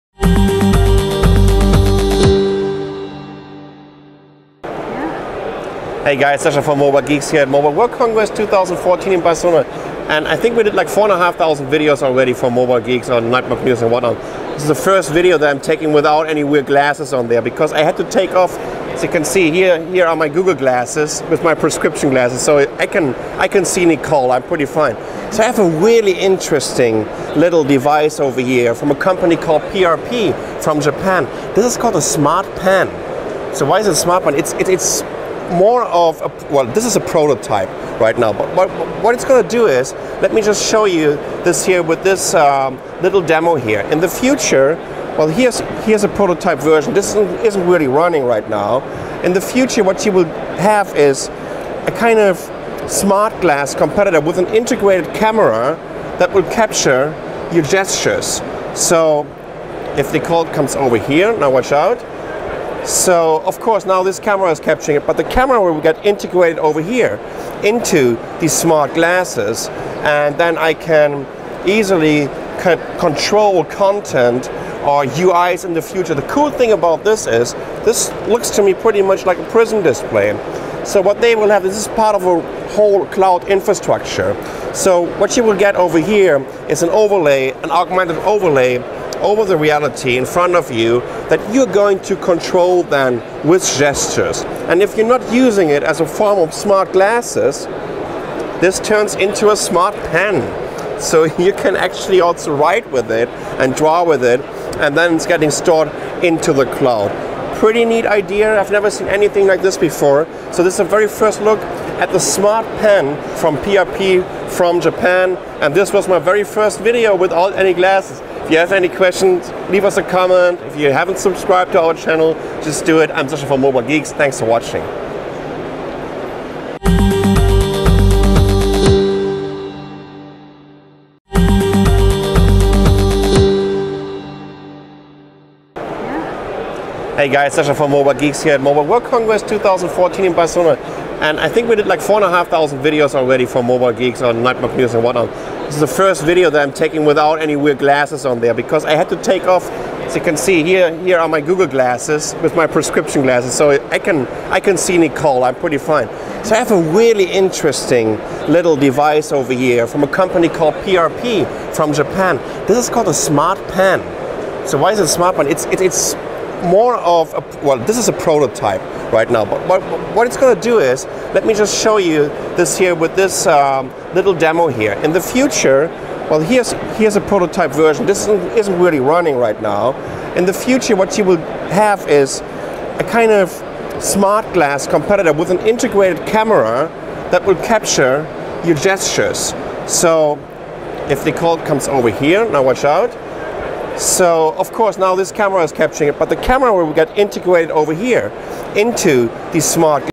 Smartpen Wearable com Controle por Gestos Mãos na MWC 2014
De vidro, com Controle por Gestos Um Japonês iniciar tem uma opinião interessante sobre wearable vidro, eles inventaram um conceito para um copo que vem com controle por gestos E ele se transforma em uma caneta para os seus dispositivos móveis.